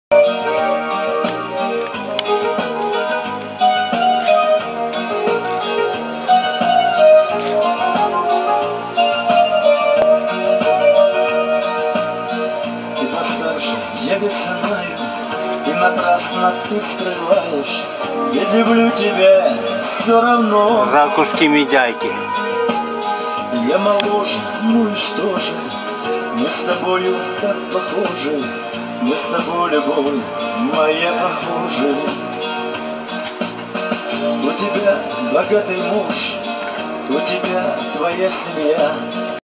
Зарисовка жизни Керчи.Набережная-пацаны пересыпают ракушки-мидяйки.
Надо было подойти ближе...да и спросить почём мидии. Ребята их на сваях пирса надрали.Есть хорошие места где мидия плотная и вкусная-а есть большая...и так себе. На набережной очень вкусная.Любимая пища детворы-да и не только...пару камней-костёр и кусок жести сверху.ОБЪЕДЕНИЕ !!
 А есть особое приготовление-плов из мидий.Закрытая-приоткрытая ракушка...и в ней плов с риса-со всеми необходимыми атрибутами.И САМОЕ ГЛАВНОЕ-МЯСО В РАКУШКИ ВЫКОВЫРИВАЕШЬ ЛОЖКОЙ СО СТЕН СТВОРОК. ЭТО ПРЯМ ПРОИЗВЕДЕНИЕ КУЛИНАРНОГО МАСТЕРСТВА.А вкуснятина...словами не передать :)Мой друг Алик-знал секреты этого пригтовления.